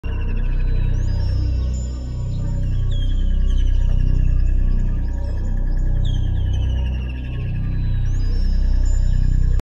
Thriller of the nature😳|wild cat
This video about wild cat rarely see in the jungle